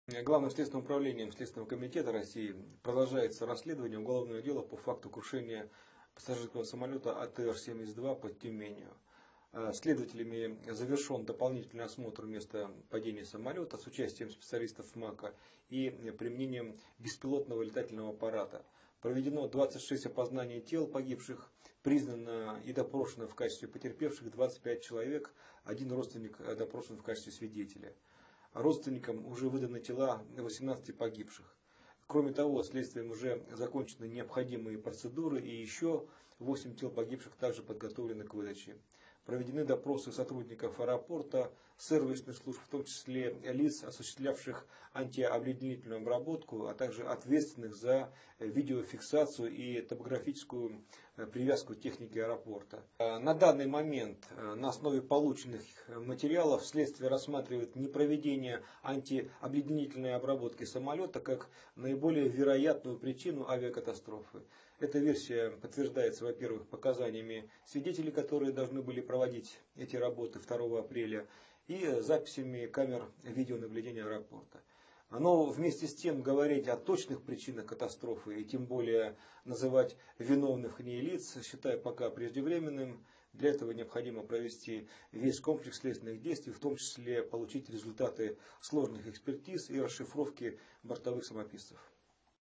April 4, 2012 Russia_ Investigators say failure to de-ice plane caused crash
Russian investigators have concluded that Monday's air tragedy was most likely caused by failure to de-ice the plane.

The ATR-72 passenger jet crashed outside Tyumen, killing 31 people.

Investigative Committee spokesman Vladimir Markin told reporters: "the investigation is considering the theory of the failure to give the plane de-icing treatment prior to the flight to be the most probable cause of the crash."

"Nevertheless, at this point in time we cannot be sure of the true causes of the crash and even less of any names of the persons guilty. In order to establish this, we need to run all the mandatory investigation procedures, and among them to obtain the results of complex expert tests and the transcript of the flight data recorders", he added.